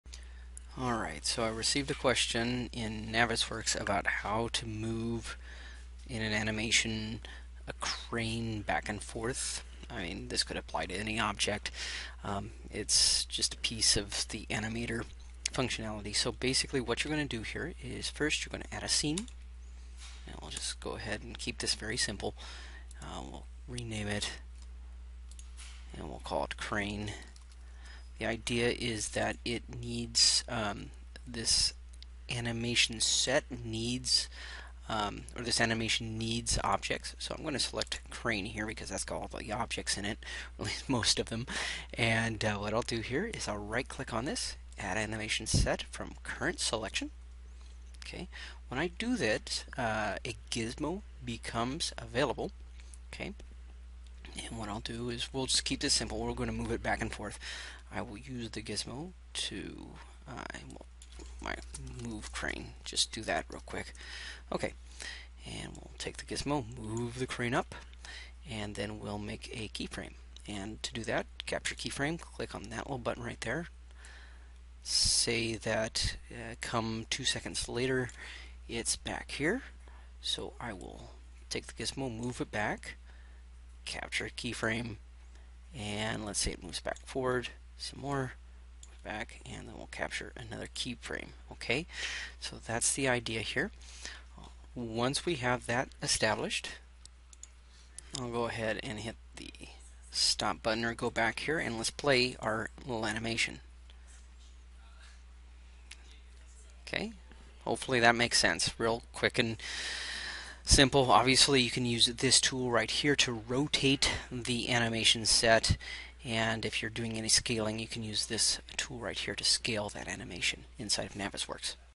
Moving a Crane in Navisworks
Quick and simple tutorial to animate a crane in a Navisworks scene.  In this case we're moving the crane.  The other tools in the Animator dialog will let you rotate and scale.